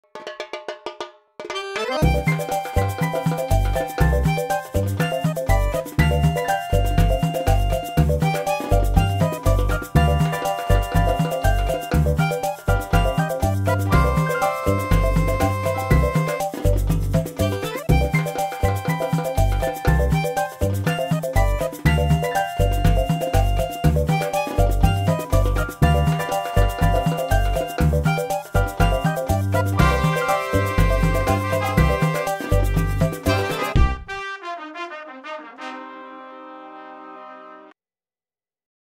THE BOOK by Marije Tolman & Ronald Tolman - Booktrailer -
THE BOOK by Marije Tolman & Ronald Tolman, published by Querido Amsterdam, oktober 2017 (book trailer by Rogier Wieland stop motion animation)